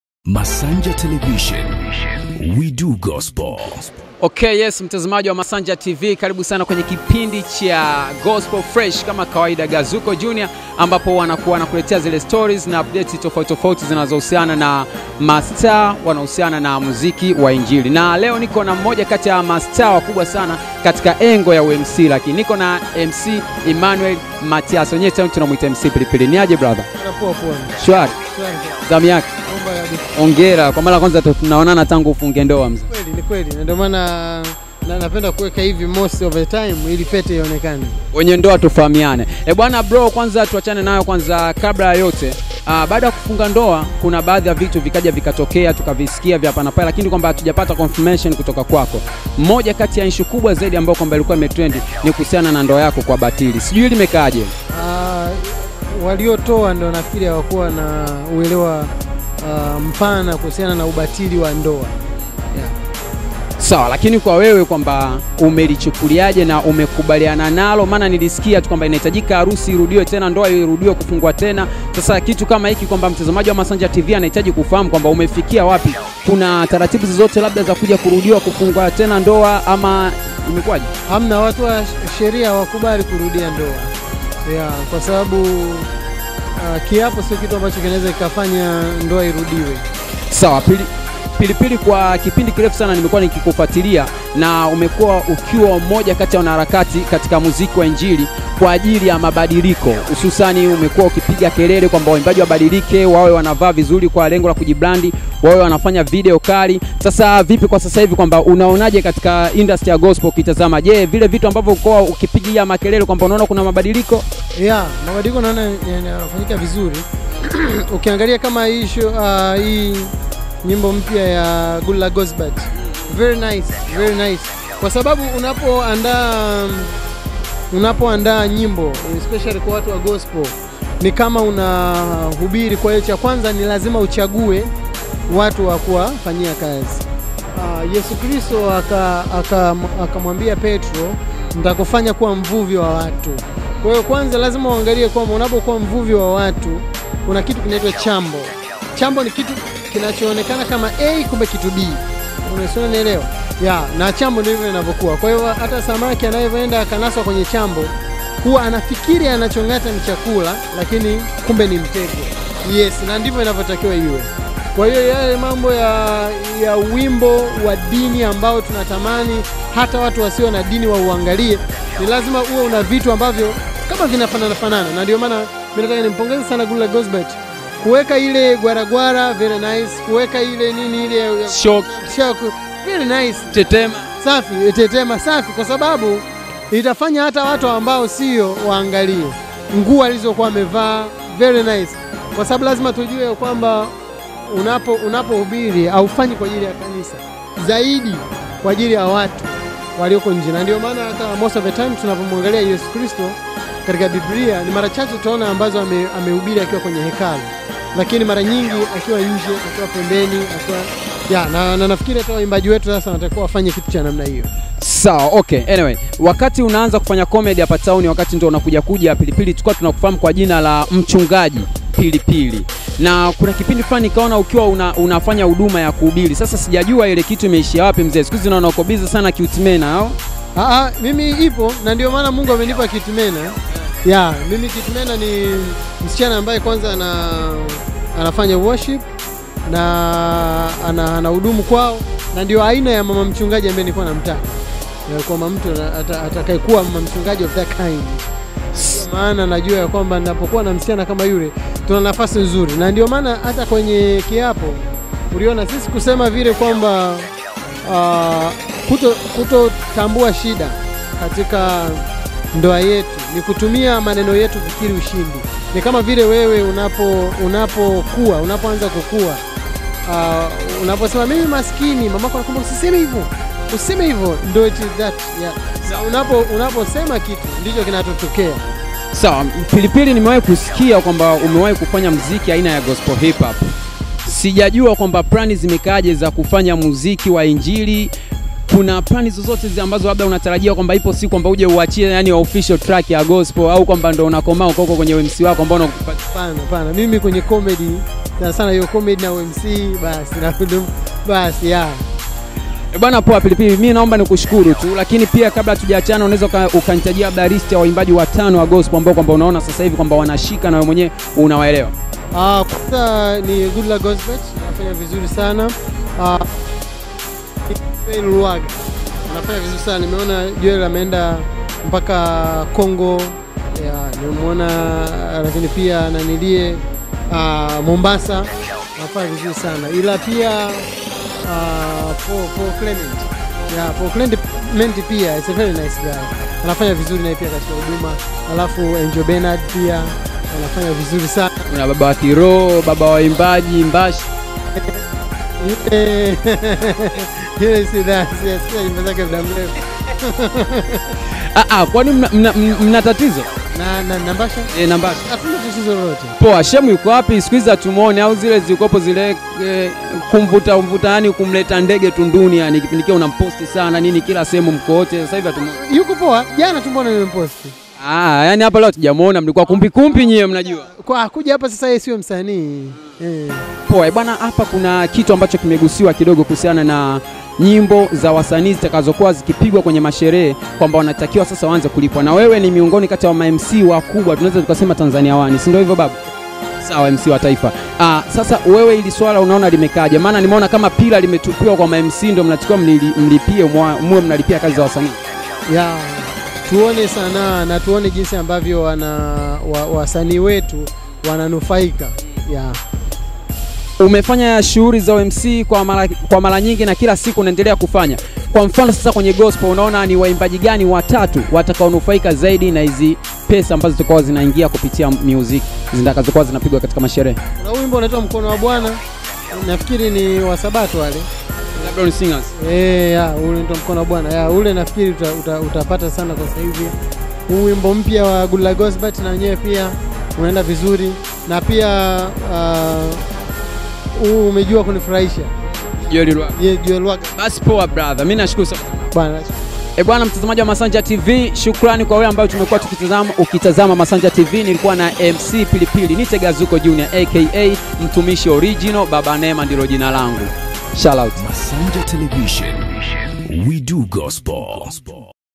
MC PILIPILI AMPONGEZA GOODLUCK GOZBERT KUFANYA MTINDO WA BONGO FLEVA| MASANJA TV
TANGAZA NASI MASANJA TV TUPIGIE SASA KWA NAMBA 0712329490